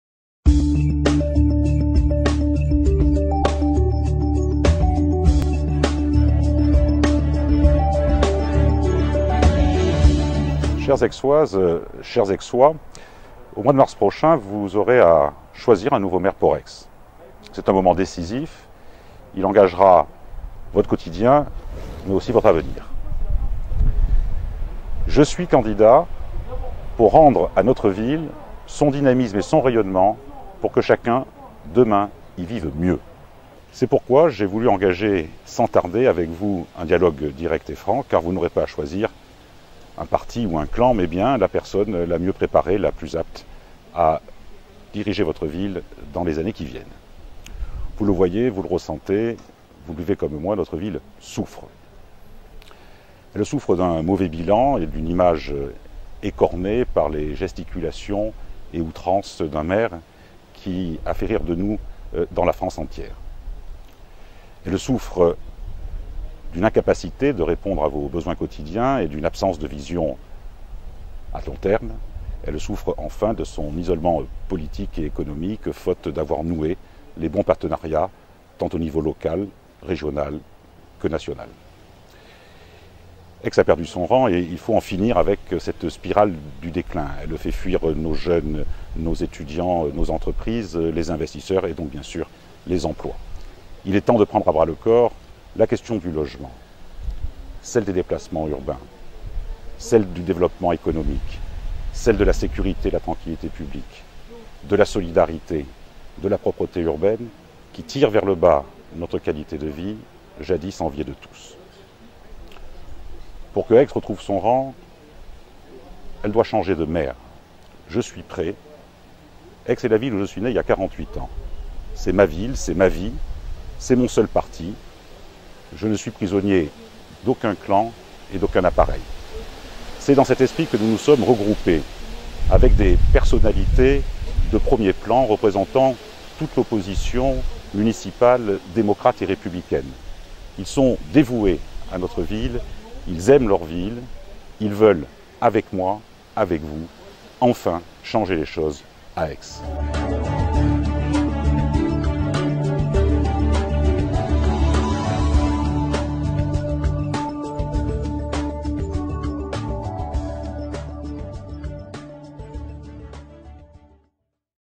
François-Xavier de Peretti s'adresse aux aixoises et aux aixois - Municipales 2014 - Aix en Provence
François-Xavier de Peretti s'adresse aux aixoises et aux aixois pour leur annoncer les raisons de sa candidature aux municipales 2014 à Aix-en-Provence.

Retrouvez la lettre aux aixoises et aux aixois sur notre

et sur twiter : @ensembleavecvou

Lieu du tournage : Pavillon Vendôme, Aix-en-Provence